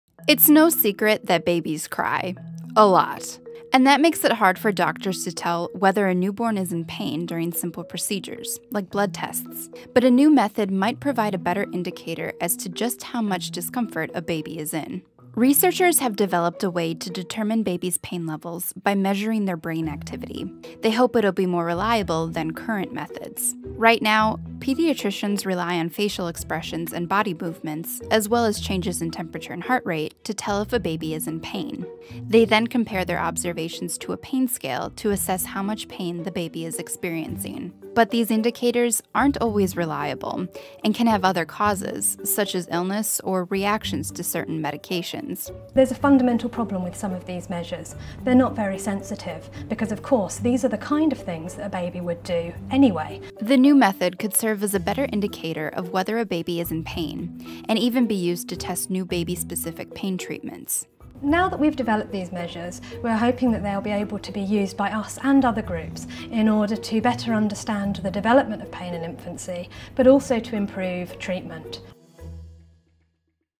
A new way to detect pain in newborns
Researchers are using brain scans to determine if babies are in pain.